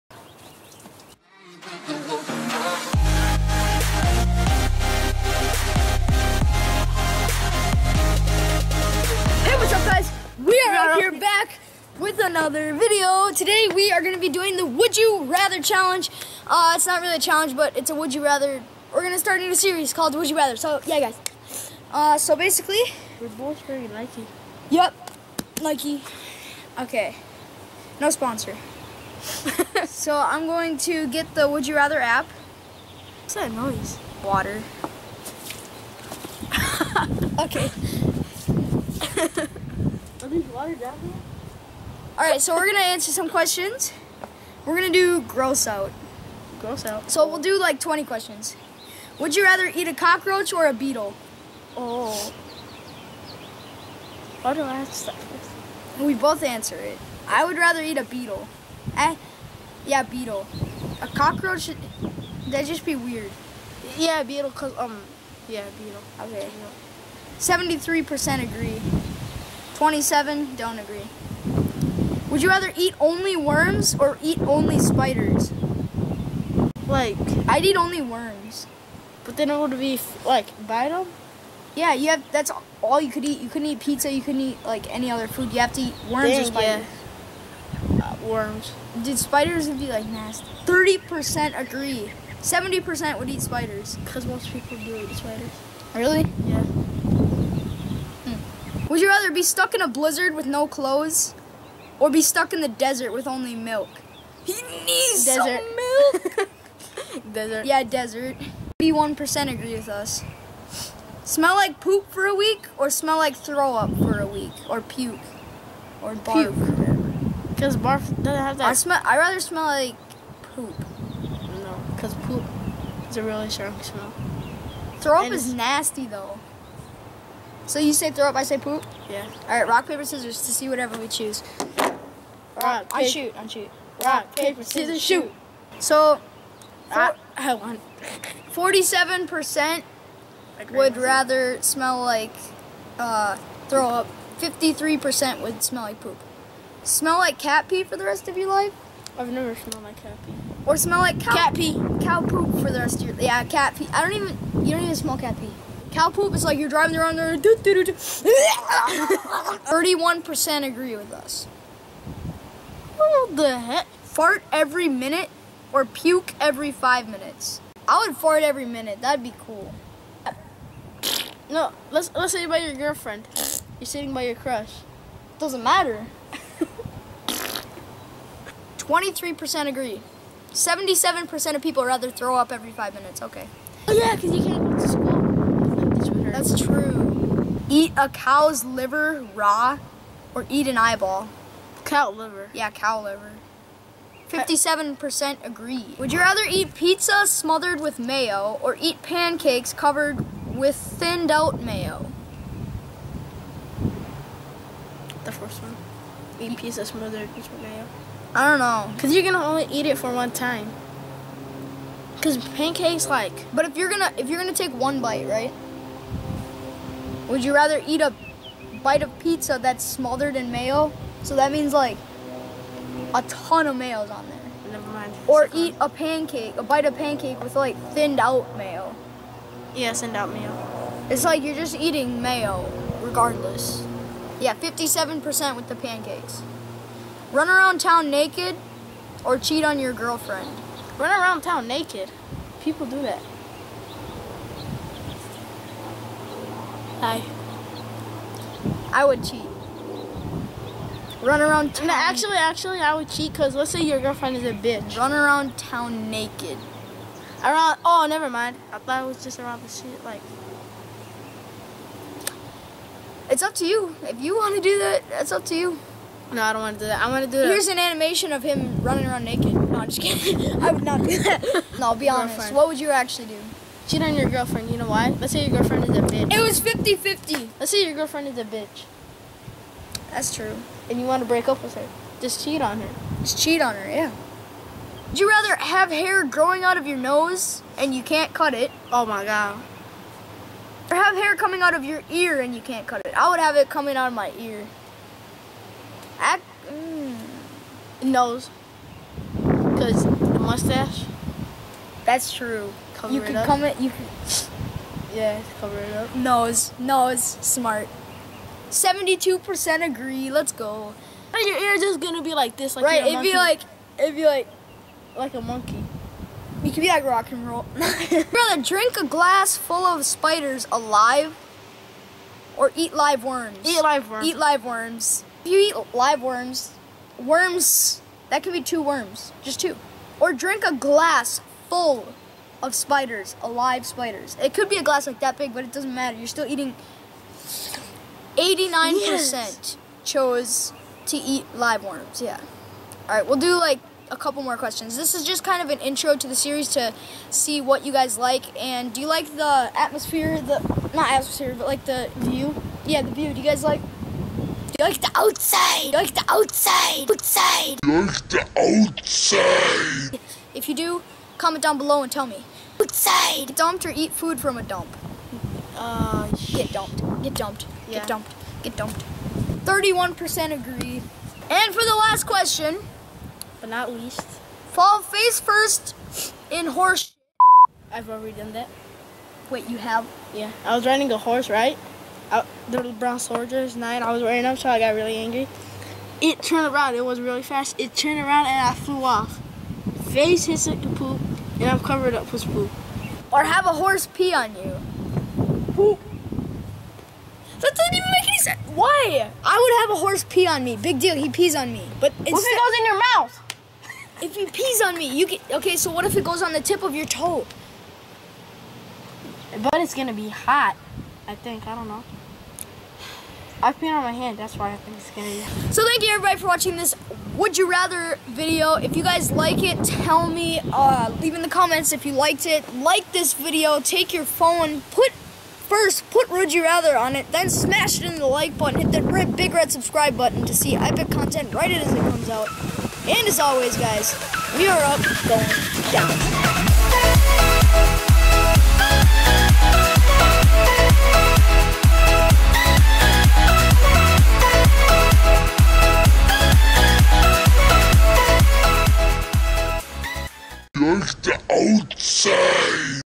RUN AROUND TOWN NAKED!? - Would You Rather
Today, Ali and Austin play some "Would You Rather!"

Follow me on Google+ to help me get my channel verified

My Clash Royale Clan: WeAreUpFanClan

WeAreUp
goin'
down